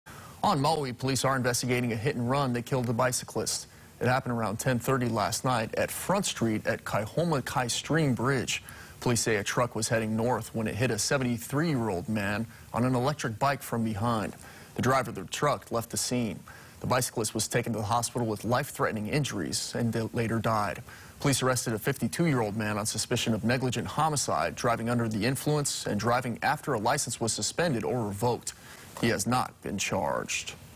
Lahaina bicyclist dies after hit and run on Front Street
A bicyclist died after a pick up truck hit him on Front Street just after 10:30 p.m. on Saturday, Jan. 29